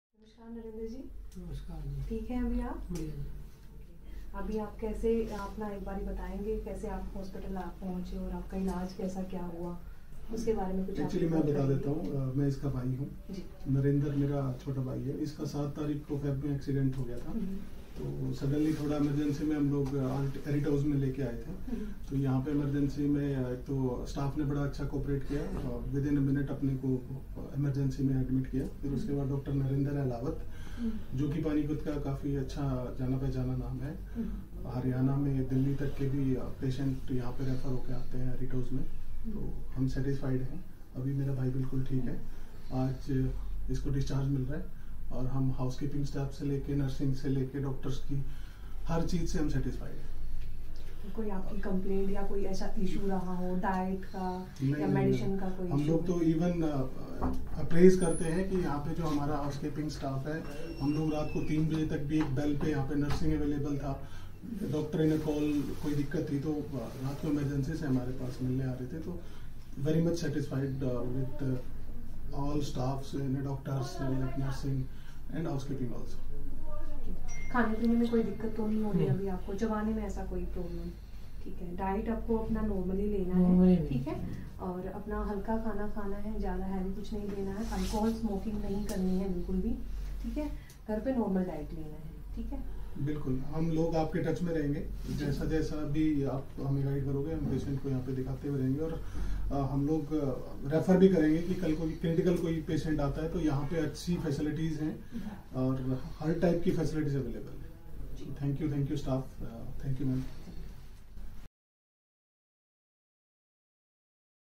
Doctor Ahlawat Restores Hope for Patient Narender and His Family After a Devastating Accident.
Patient Narender was admitted to the hospital after a terrible accident. His family was devastated and worried about his health. But, their worries were allayed when they met Doctor Narender Ahlawat.

Doctor Ahlawat, with his extensive experience and qualifications, was entrusted with the responsibility of treating Narender. He was thorough and meticulous in his diagnosis and treatment. He also took into consideration the comfort and safety of the patient and his family. He made sure that the right medicines were administered, the right tests were conducted, and the correct diagnosis was confirmed.

Narender’s family was immensely satisfied with the professionalism and care of the doctor and the hospital staff. The housekeeping staff and nurses were also friendly and attentive, making sure that Narender was comfortable throughout his stay.

The family was also thankful for the support and guidance provided by the hospital staff, as well as for the overall quality of care that Narender received. With the help of Doctor Ahlawat and the other members of the hospital staff, Narender was able to make a complete recovery from his accident.

In the end, Narender’s family was relieved and grateful for the excellent care that Narender received during his stay at the hospital. The kindness, professionalism and attention of the doctor, housekeeping staff, and nurses were truly appreciated. They were great support during Narender’s recovery and made sure that he was in the best of health.

सटीक और सही ईलाज के लिए

"Artios है ना"

✅ Our Address - Before Toll, Near Tau Devi Lal Park, On G.T. Road, Panipat 132103